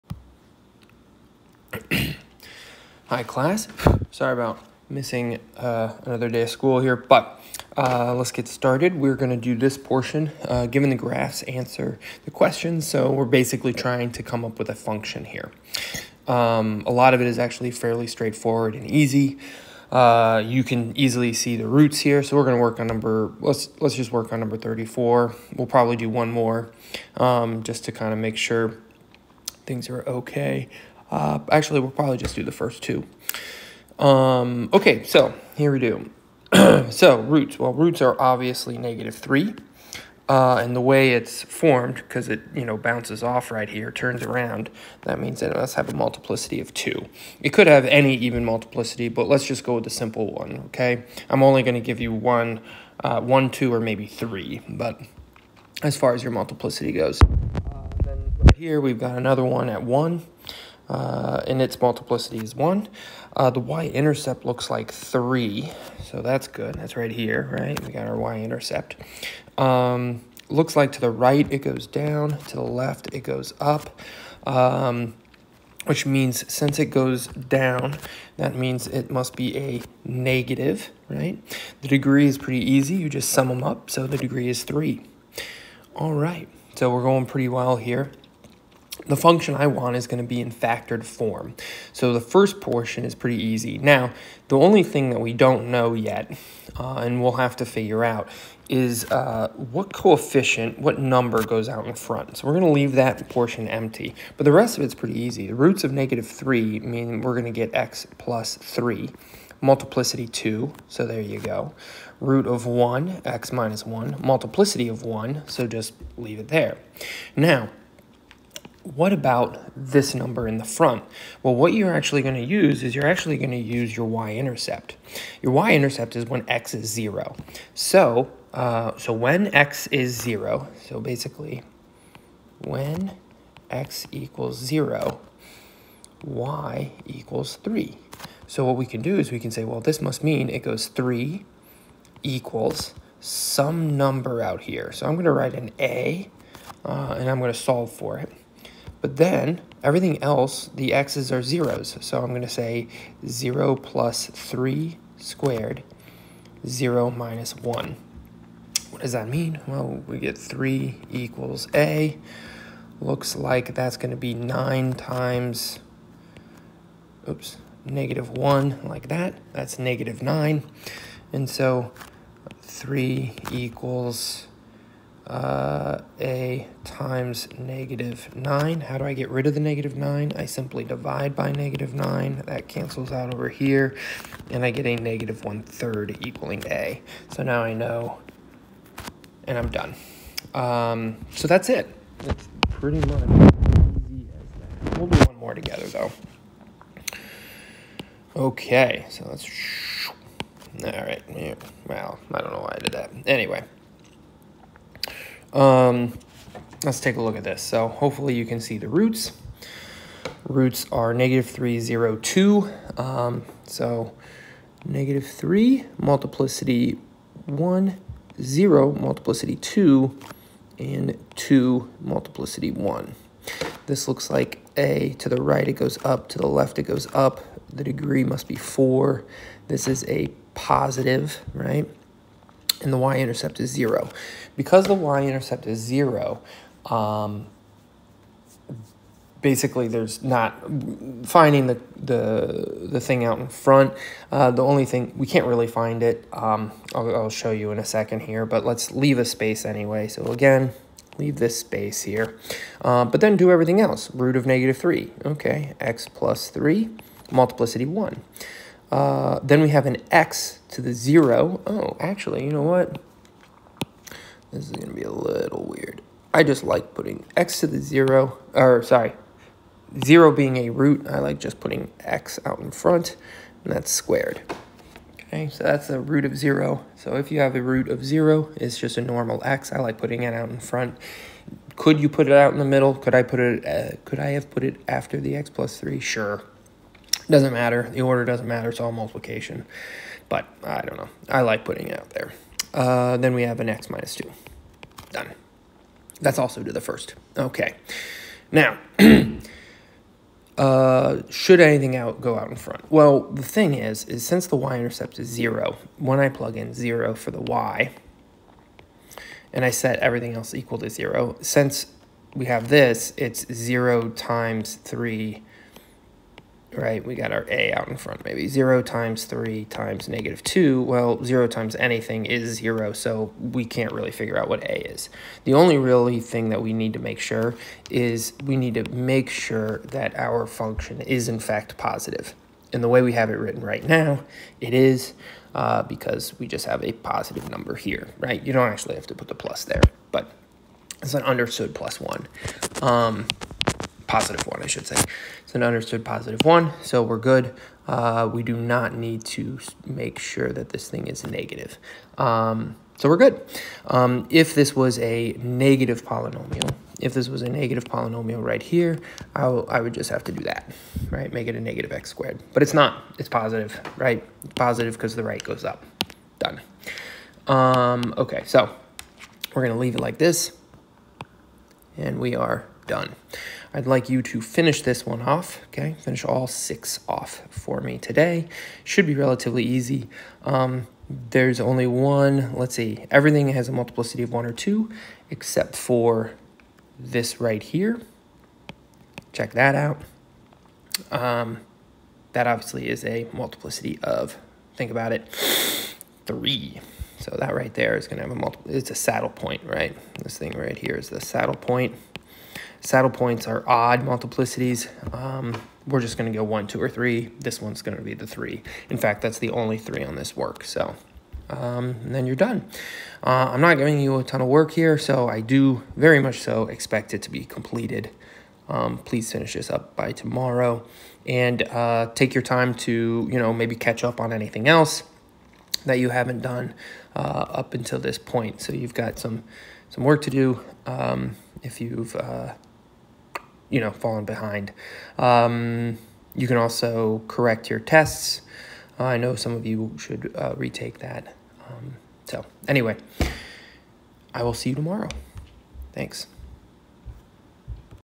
October 1, 2024
Modeling polynomial function based on graph.